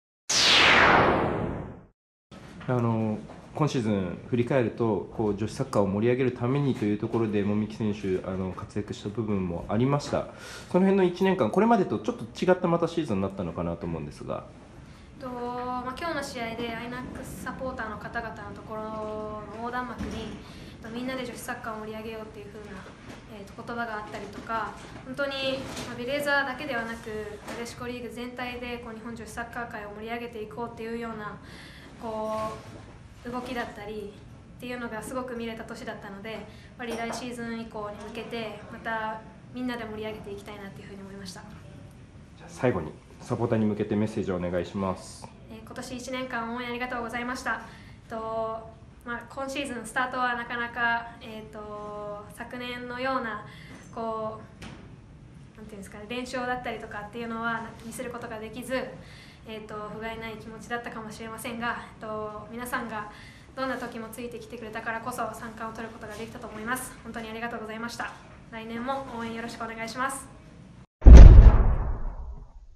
I神戸戦／籾木結花選手試合後インタビュー後編
[MATCH MOVIE] after the match against INAC KOBE LEONESSA
INAC神戸レオネッサ戦、籾木結花選手の試合後インタビュー後編をお届け！

◆皇后杯 JFA 第40回全日本女子サッカー選手権大会 決勝
日テレ・ベレーザ 4-2 INAC神戸レオネッサ